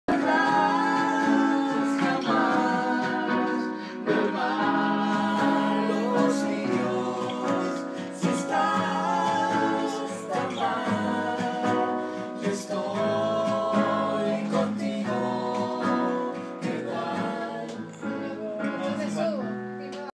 Tomás Betín grabando nuevo álbum 2013
Grabando en algún lugar de Chapinero, con el Jose, Lego y Ana.